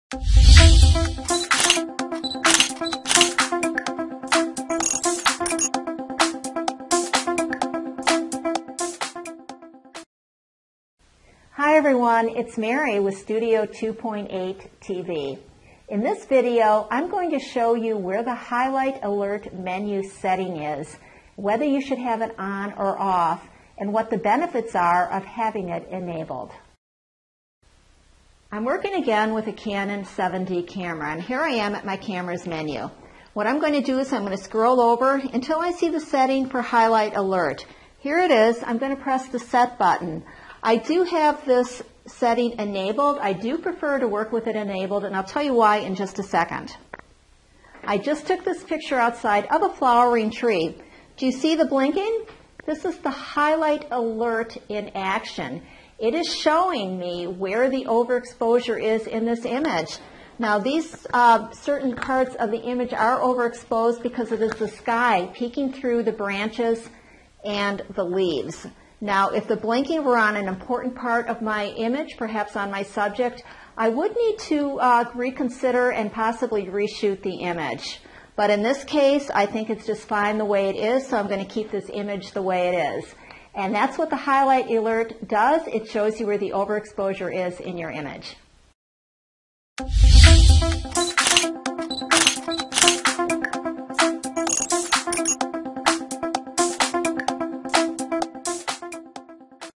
Digital Camera Highlight Alert Menu Setting on a Canon Camera.
Digital camera highlight alert setting on a Canon digital camera is shown with an explanation of what the advantages are of having it enabled.